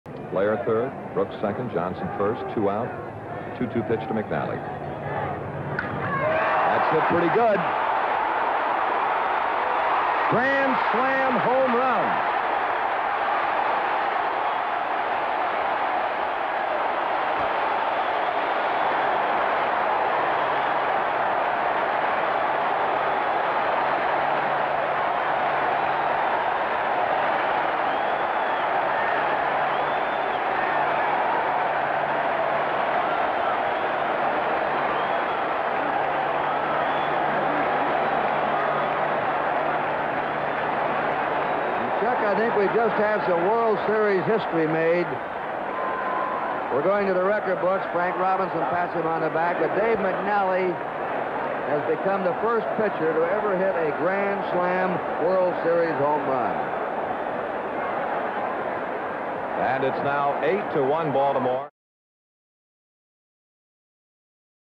WS Gm3: McNally the first pitcher with slam in Series
10/13/70: Orioles pitcher Dave McNally is the first pitcher ever to hit a grand slam in a World Series game